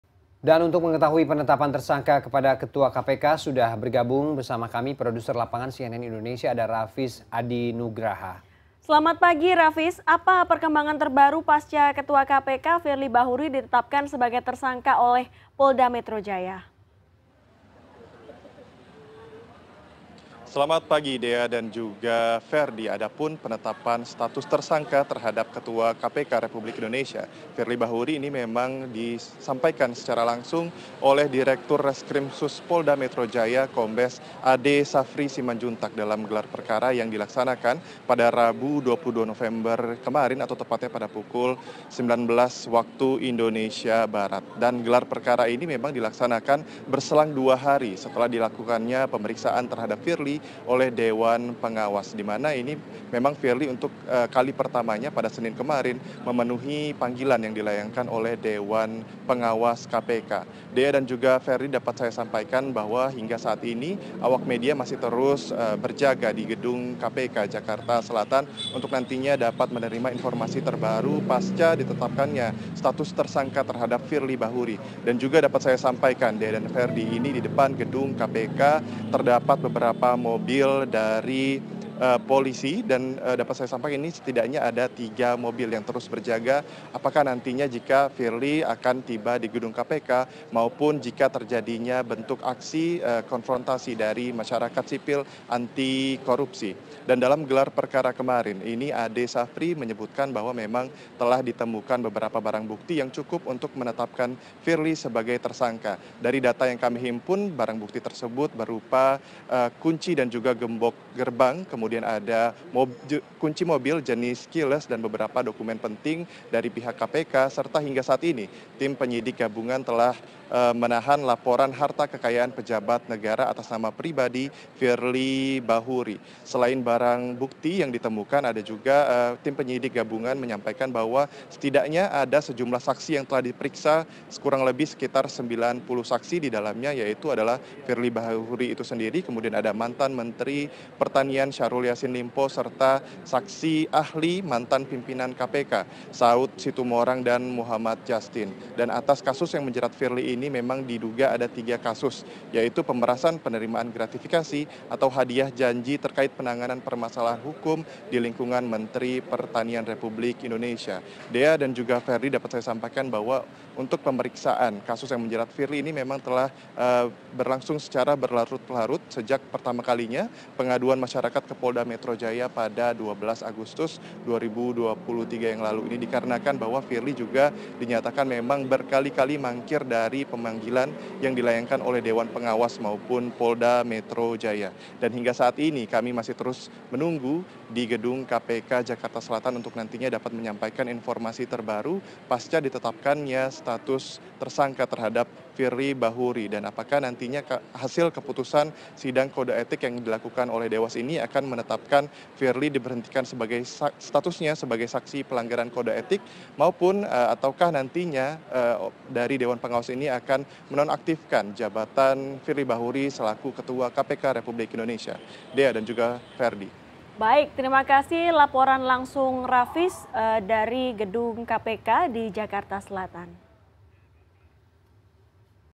Ketua KPK Resmi Jadi Tersangka Kasus Pemerasan
Dan untuk mengetahui penetapan tersangka kepada Ketua KPK sudah begabung bersama kami produser lapangan CNN Indonesia, Rafiz Adi Nugraha.